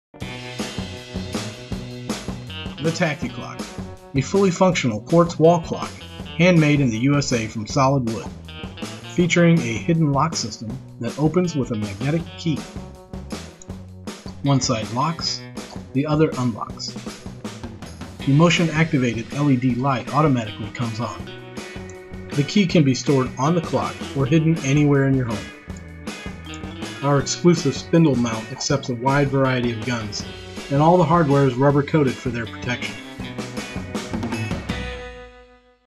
Original Tacti-Clock Concealment Wall Clock Gun Safe with Hidden Magnetic Lock
The Tacti-Clock is a fully functional quartz wall clock with a hidden lock mechanism that is opened with a magnetic key. Handmade in the US from solid wood, it is safe and secure yet allows for quick and easy access. Several styles and options available.